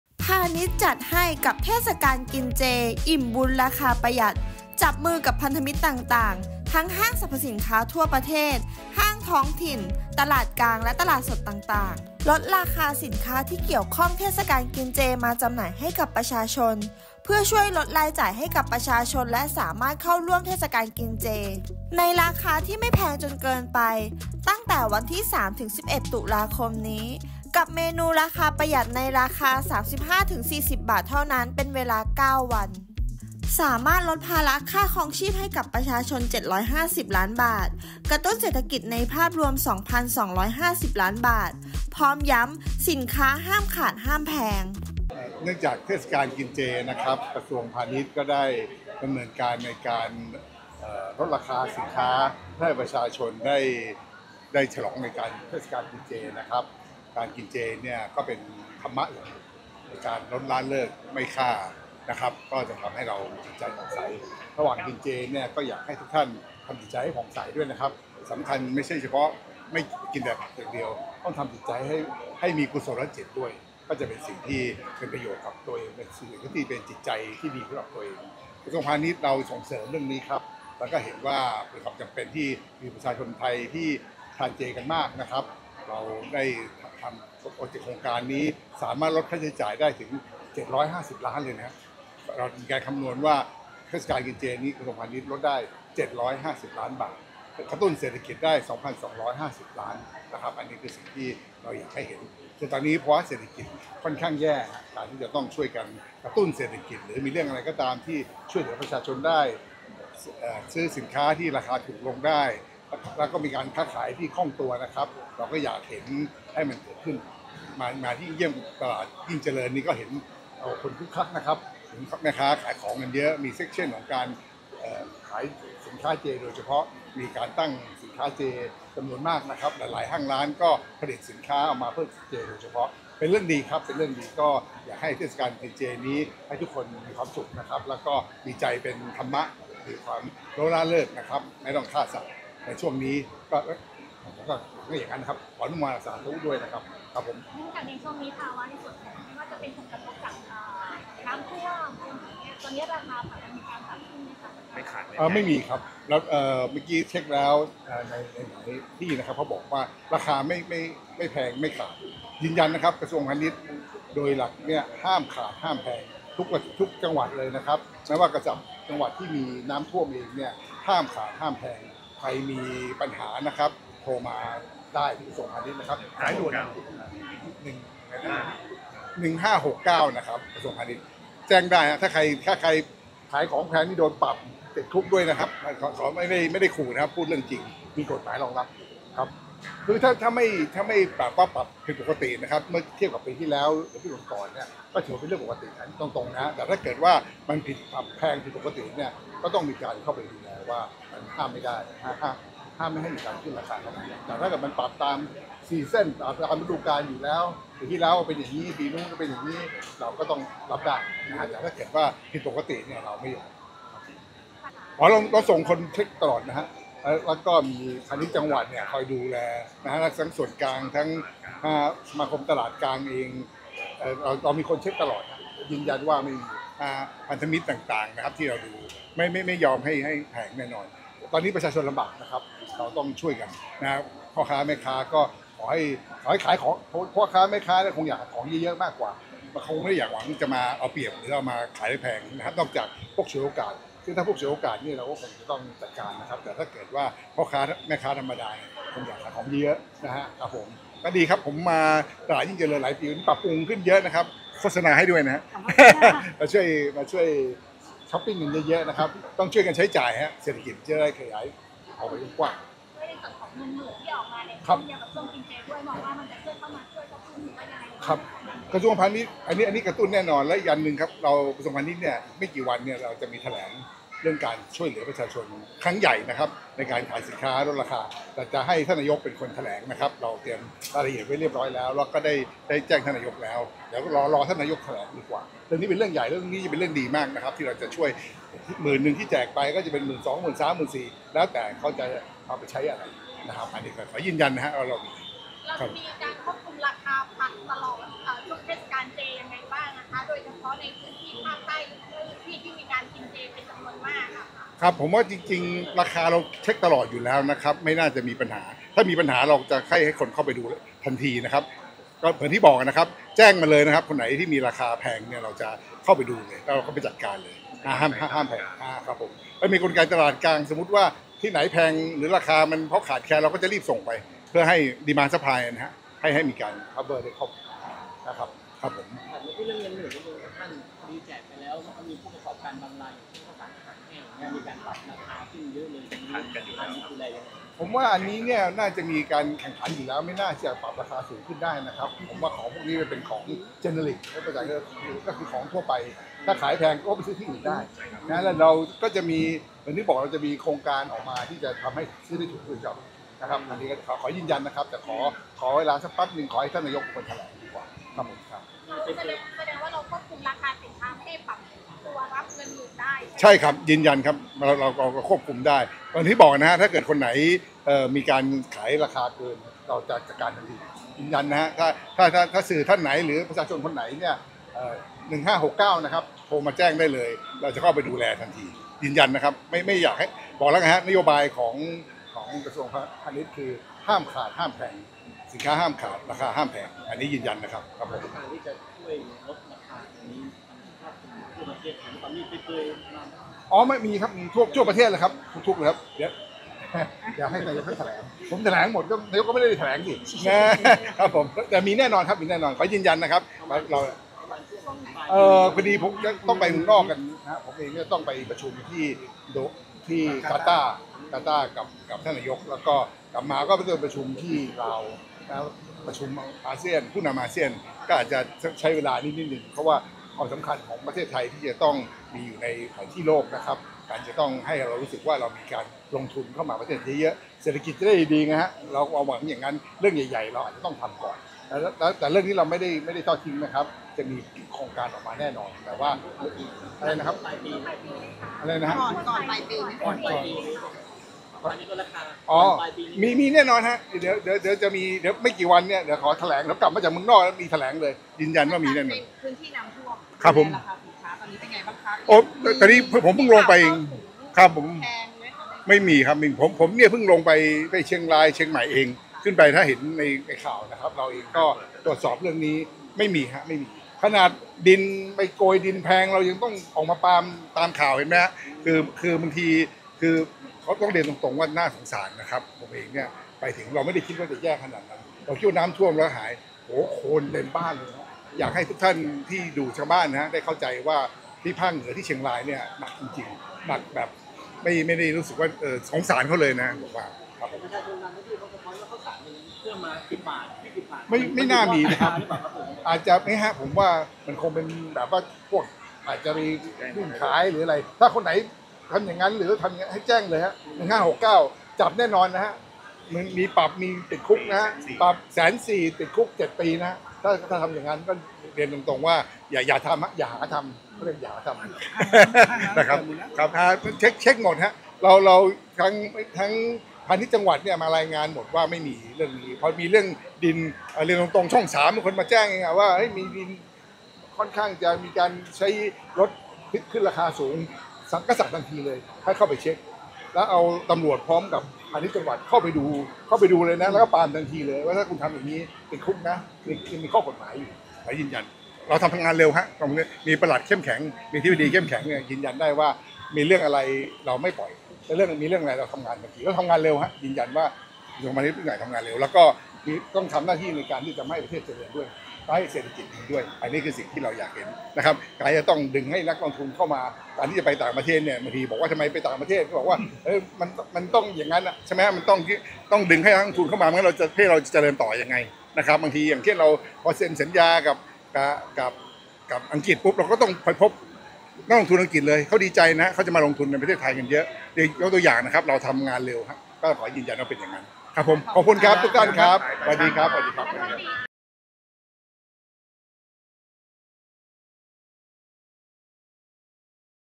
"พาณิชย์" ลดราคาสินค้าเทศกาลกินเจ ย้ำ “ห้ามขาด ห้ามแพง” คาดกระตุ้นเศรษฐกิจ 2,250 ล้านบาท
“พาณิชย์จัดให้ ลดราคา เทศกาลกินเจ อิ่มบุญราคาประหยัด” ในเทศกาลกินเจ ตั้งแต่วันที่ 3 - 11 ตุลาคม 2567 จำนวน 9 วัน สามารถลดภาระค่าครองชีพให้กับประชาชน 750 ล้านบาท กระตุ้นเศรษฐกิจ ในภาพรวม 2,250 ล้านบาท พร้อมย้ำสินค้าห้ามขาดห้ามแพง โดยเฉพาะในพื้นที่น้ำท่วมภาคเหนือ กระทรวงพาณิชย์พร้อมดูแลและตรวจสอบสถานการณ์อย่างใกล้ชิด